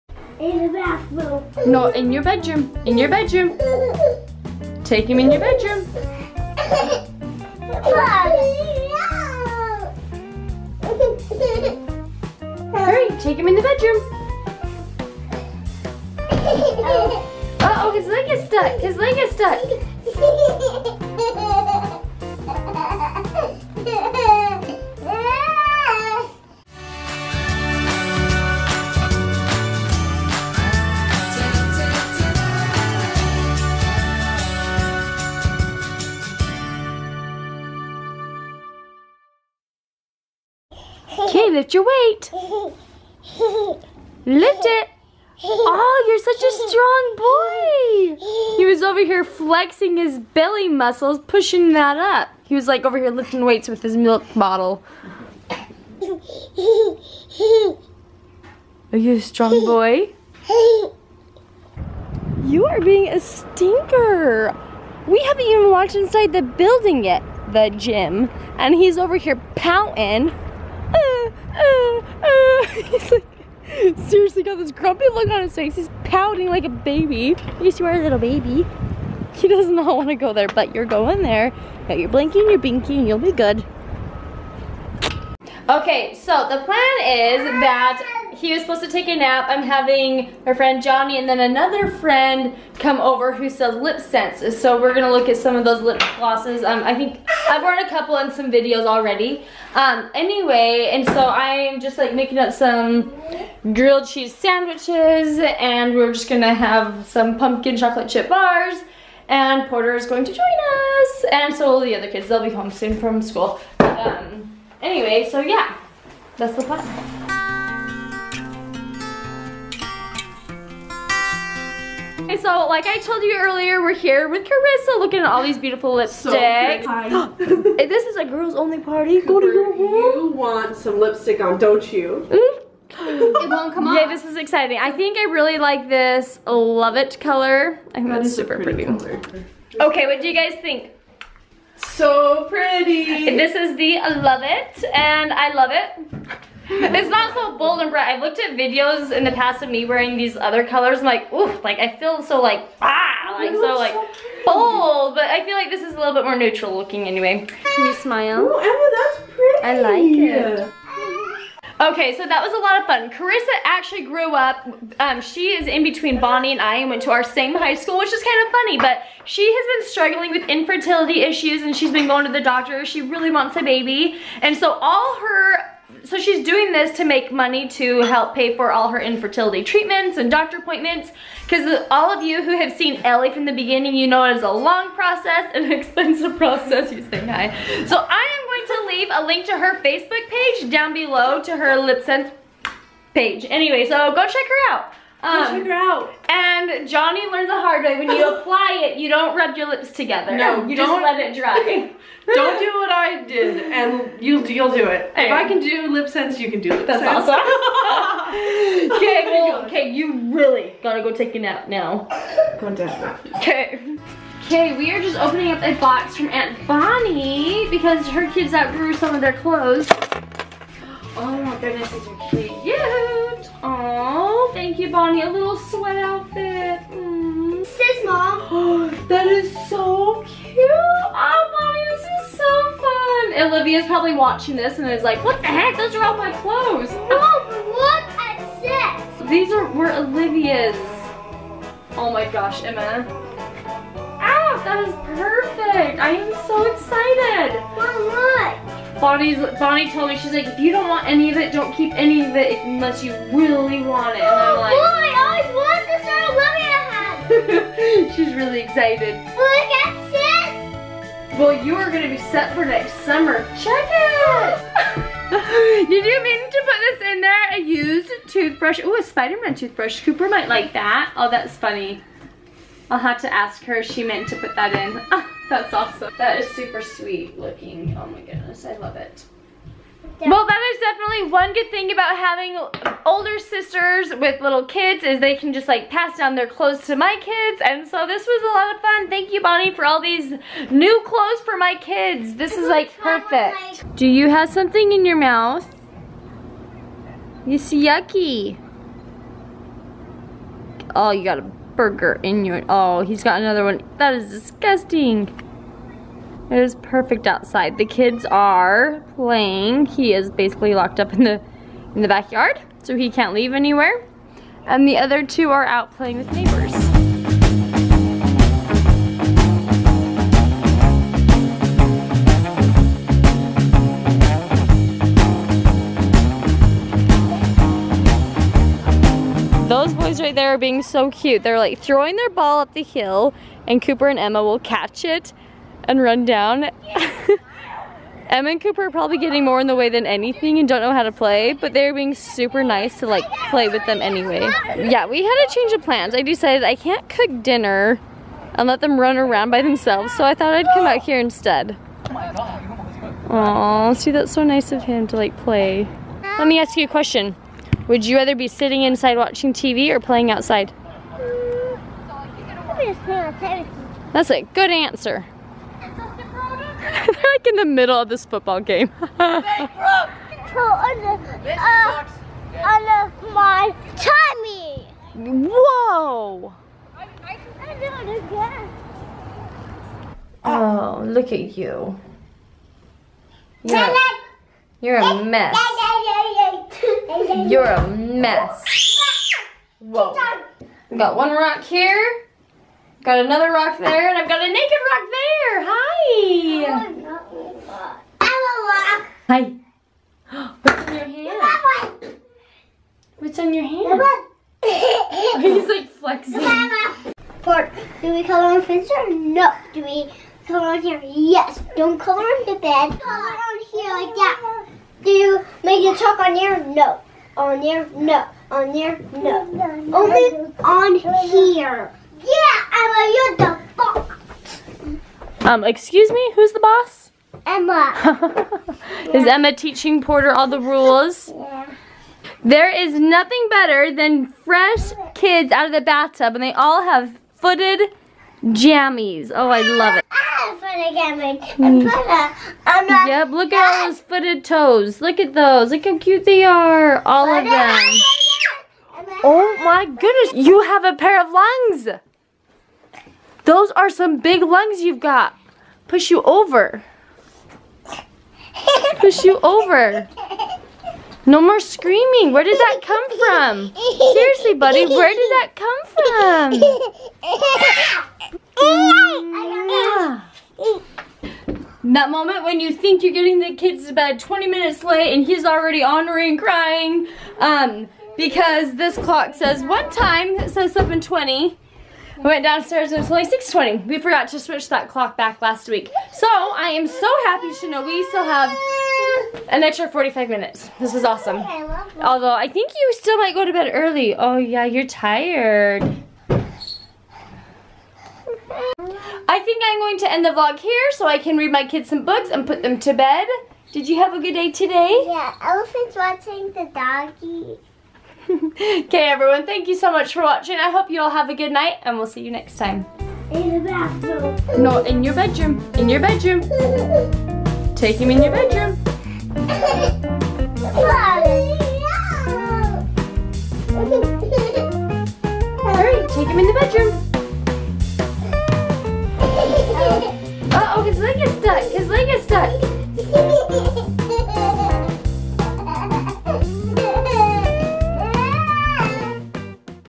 Surprise Found in a Package ~ Thanks Aunt Bonnie
My kids got a fun package in the mail from Aunt Bonnie and Olivia. I probably don't need to buy any more clothes for a while. or a toothbrush 😂
Also, my friend is selling lipSense to help pay for infertility appointments. Seriously amazing lip colors!

              (OR)
Riverside, MO 64168